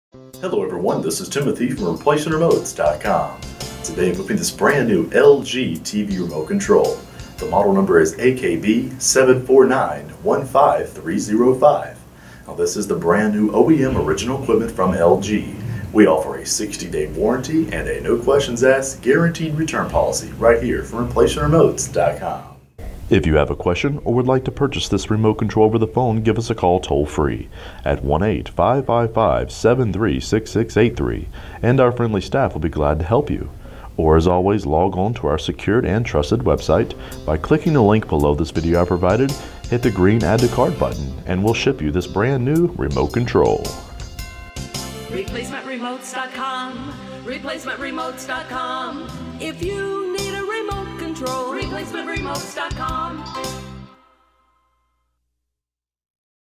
LG AKB74915305 TV Remote Control
**NOTE** Prices in the video are subject to change at any time.
- R.R. STAFF **Click below to order**

Product description

The original LG AKB74915305 Remote Control carries a 60-Days Warranty.

Model: AKB74915305
Part Number: AGF76631052
UPC: LG ELECTRONICS
Catalogue No.: 90557
This remote control controls units : TV

Compatible with TV models: 43LH5700UD, 43UH6100, 43UH6100UH, 49LH5700UD, 49UH6100UH, 49UH6500UB, 55LH5750UB, 55UH6150UB, 55UH6550, 55UH6550UB, 60UH6550UB, 65UH6150UB, 65UH6550UB
Note: This list may not be a complete list.